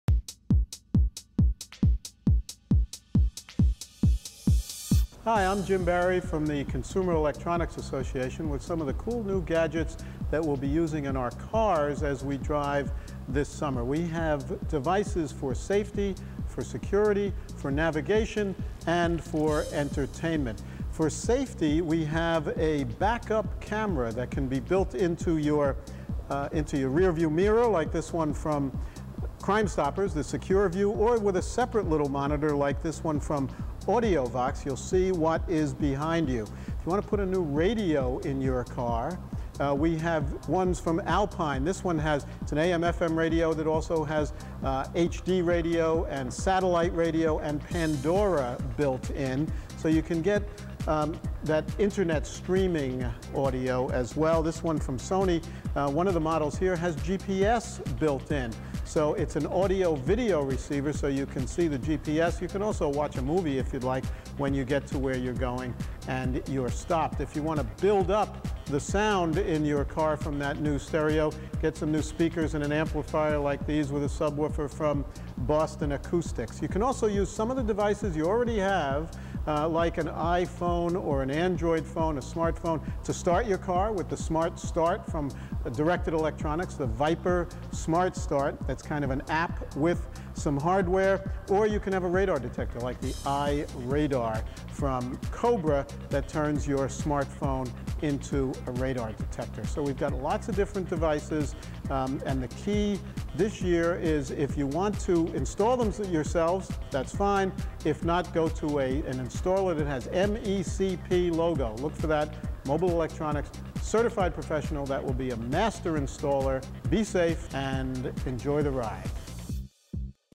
Jim Barry Shares Latest Tech Gadgets For Your Car
Jim Barry of CEA is a technology expert.  Here he talks shop regarding the latest technology for your vehicle from safety to keeping the kids entertained on the road, and showcases innovative and cool new gadgets for the road.  Video by CEA.